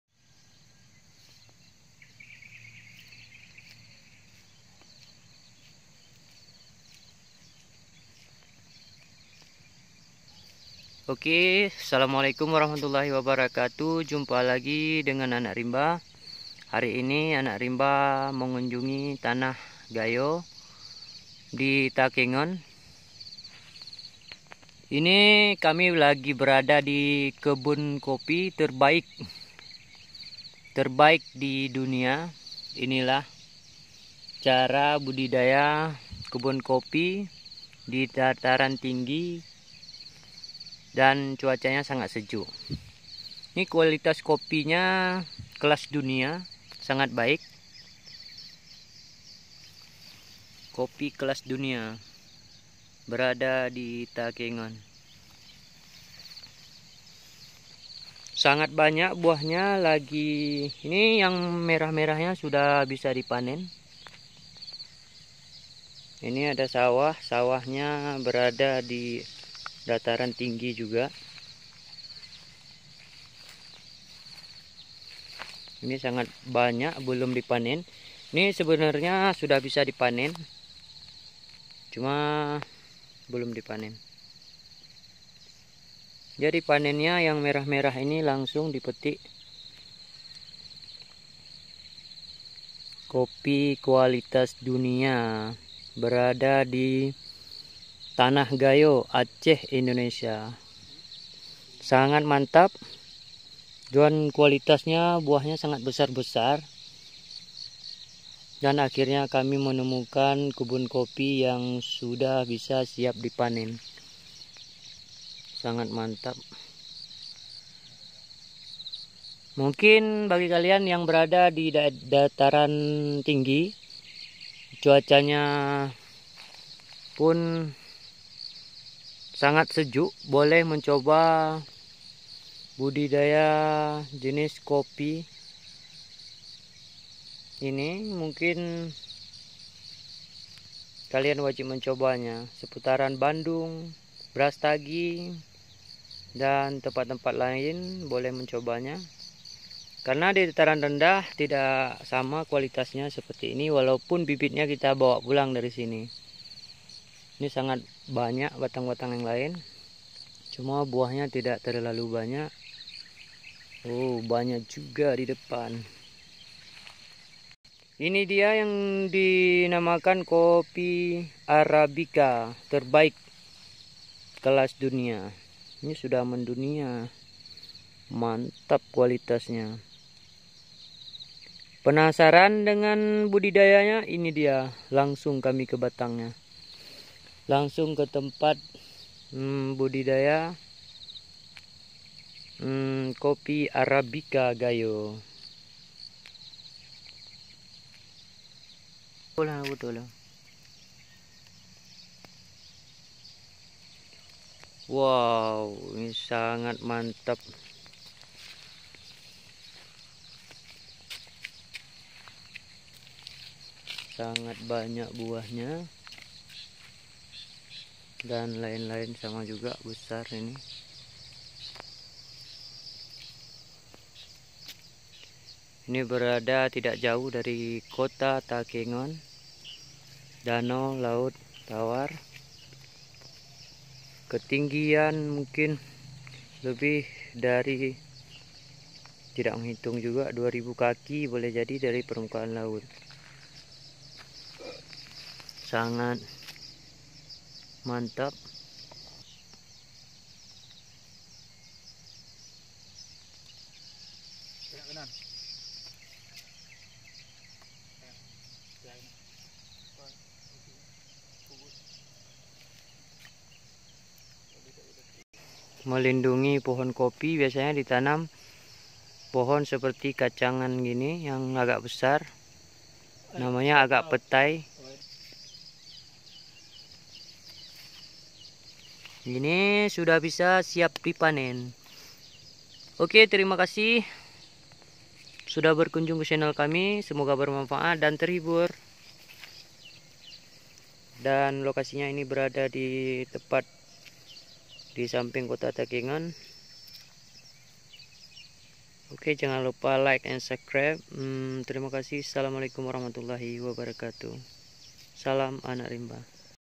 Amazing ! Coffe Arabica Gayo ! Budidaya Kopi Arabica Gayo ! Kopi Terbaik No.1 Di Dunia
Coffe Arabica Gayo ! Kopi Arabica Gayo ! Kopi Terbaik No.1 Di Dunia

Yok Lihat Langsung Kebun Kopi
Terbaik Di Dunia
Kualitas Super Masbro...

Gayo.Takeungon
Aceh.Indonesia